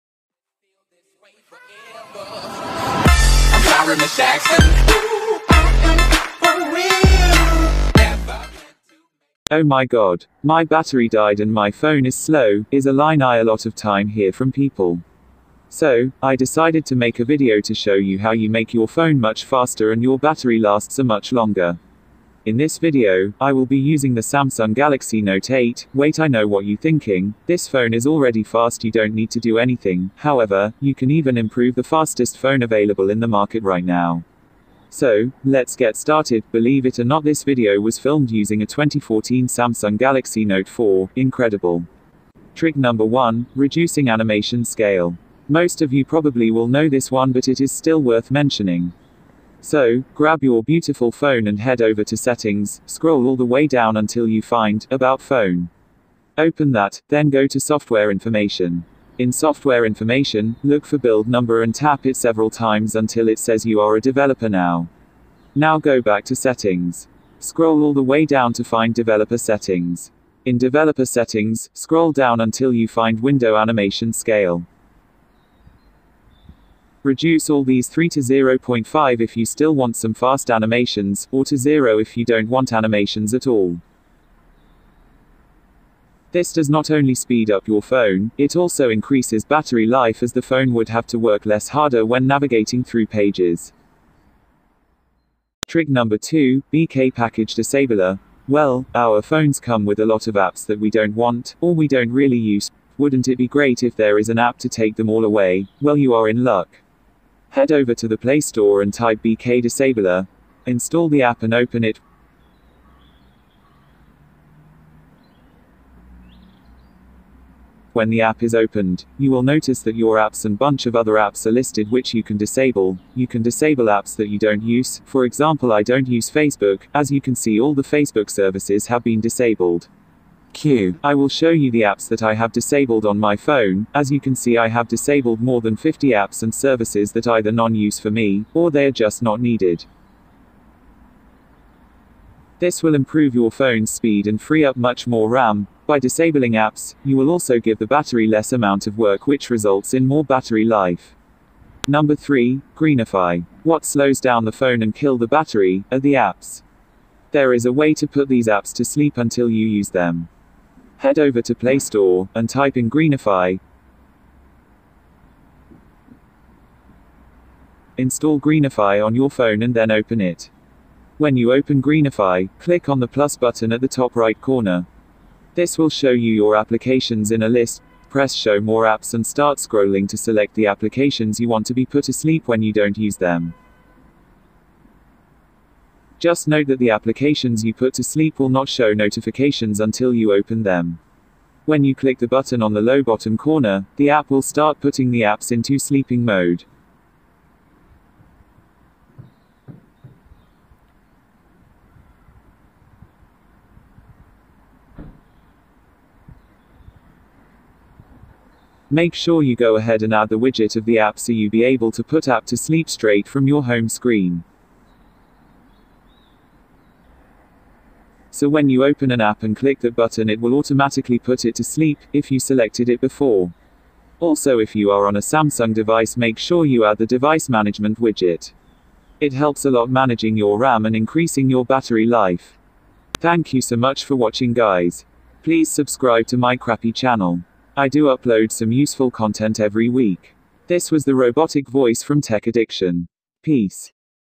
Increase Battery Life Note 8 - Speed Up Your Phone Note 8
NEW UPLOADS-

Here is how to speed up your phone and make your phone 10 times faster. This also increases phone battery life.
The steps I explain in the video help you speed up your slow phone and it even speeds up fast phones. You can speed up your smartphone easily without a hassle.  I have demonstrated this video on speed up samsung phone the galaxy note 8.

The techniques I use also increase android phone battery life by 25%.

recording using Samsung Galaxy Note 4

Phone used Samsung Galaxy note 8